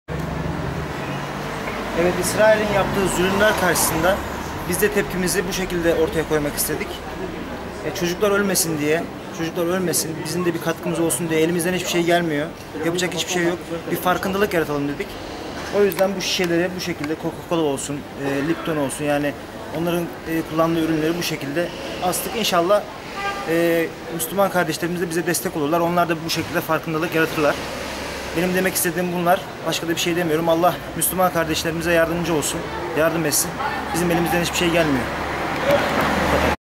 Trabzonlu'dan Protesto
istanbul güngören kınalı caddesi üzerinde esnaflıkyapan Trabzon maçkalı onur Aydın  işyerinin önündeki ağaçlara astığı kola şişelerinin üzeriine yazılar yazarak israili pretesto edip müslümanların uyanmasını ve allahım müslümanlara yardım et dğiye dua ediyor